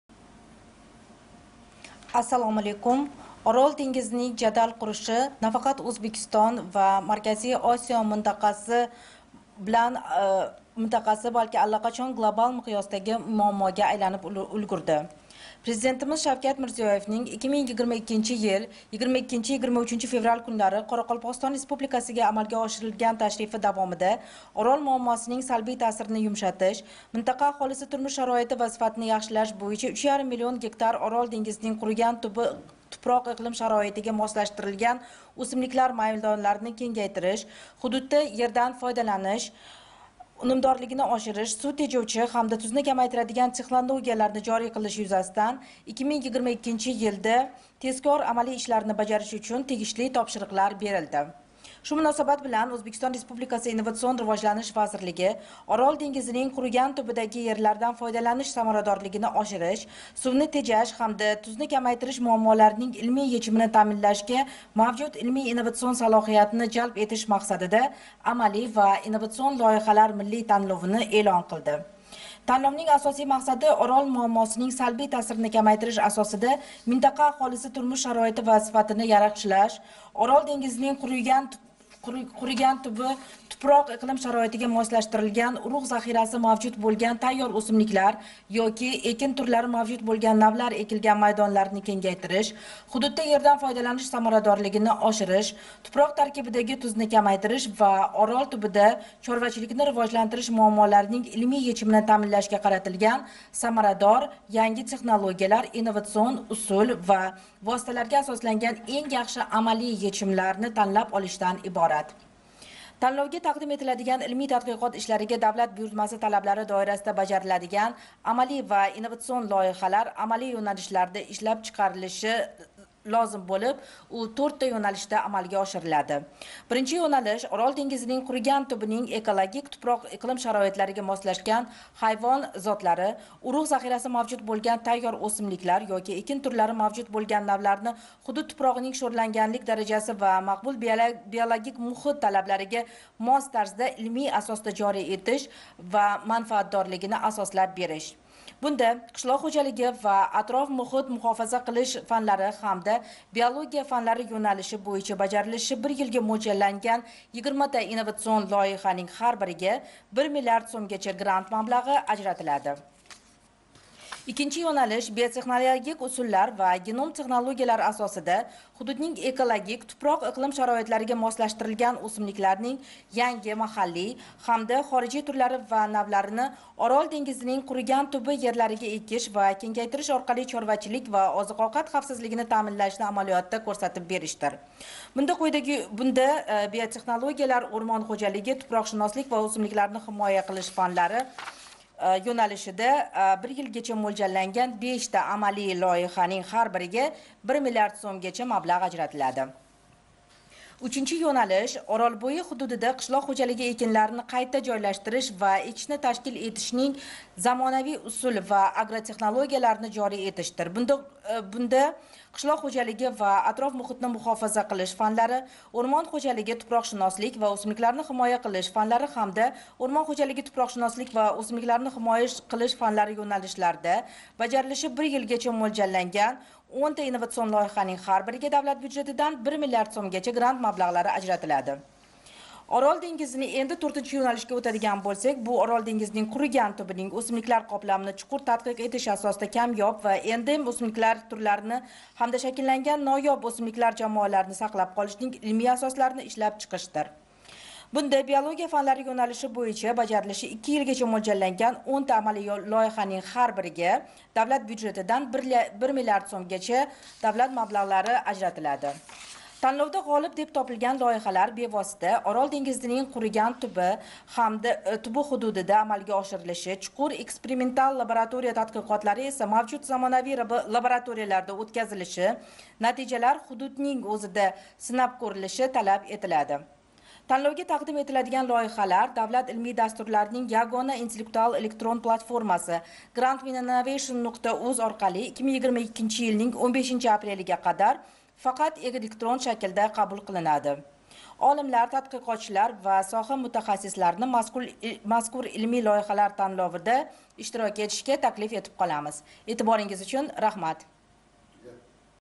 Ilmiy loyihalar Milliy tanlovi (Brifing)
Orol dengizining qurigan tubidagi yerlardan foydalanish samaradorligini oshirish, suvni tejash hamda tuzni kamaytirish muammolariga qaratilgan ilmiy loyihalar Milliy tanlovi haqida

● Vaqti: 12:00

● Ishtirok etadi: Nazokat Abduqunduzova, Innovatsion rivojlanish vazirligi axborot xizmati rahbari